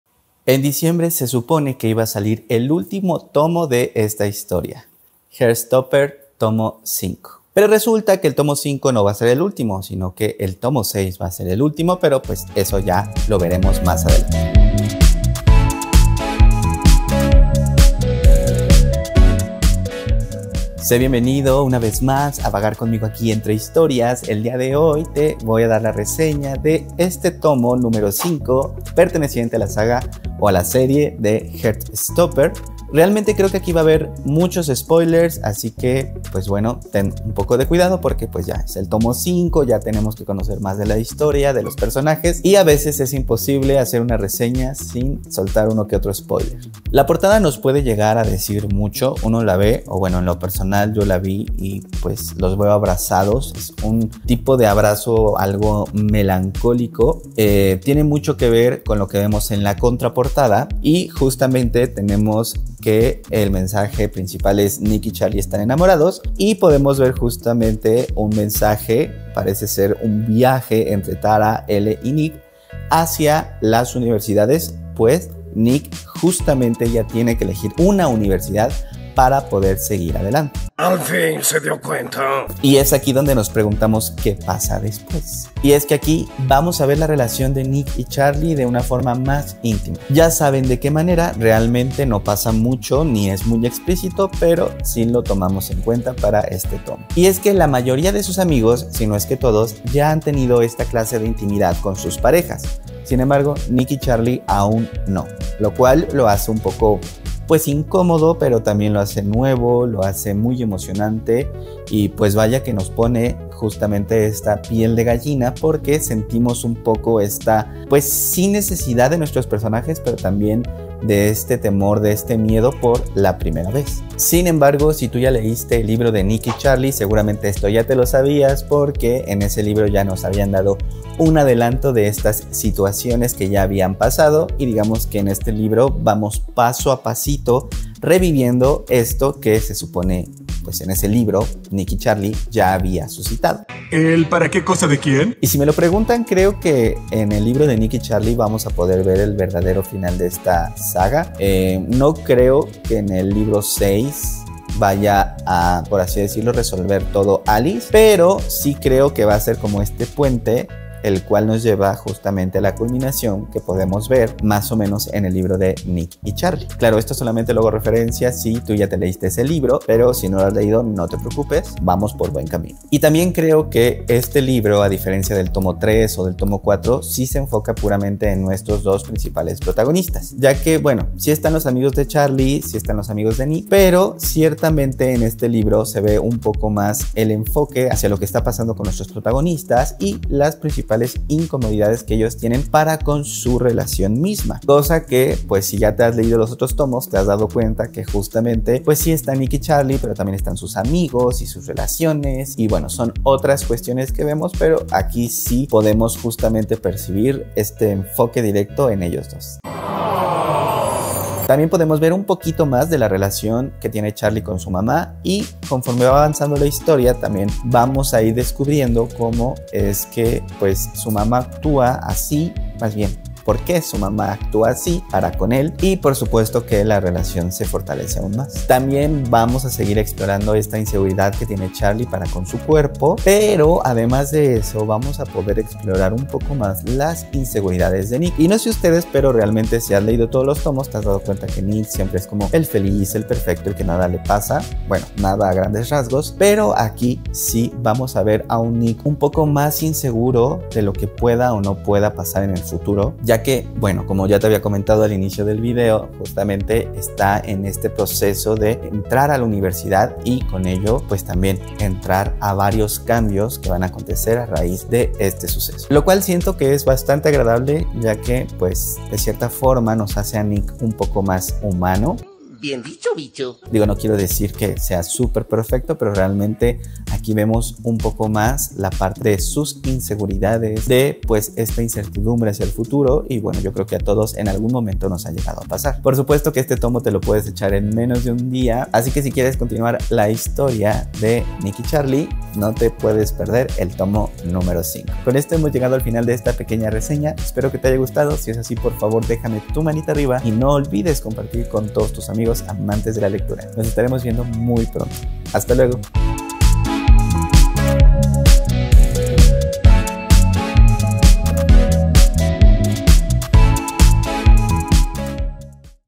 Heartstopper 5 - Alice Oseman RESEÑA
¡Hola, amigos de Entre Historias! Hoy les traigo una reseña del quinto libro de la serie Heartstopper: "Heartstopper: Tomo 5" de Alice Oseman. 🌟📚 Esta entrega continúa la historia de amor entre Charlie y Nick, llevándonos a nuevos desafíos y momentos entrañables.
Espero que disfruten "Heartstopper: Tomo 5". ¡Nos vemos en el próximo video de Entre Historias!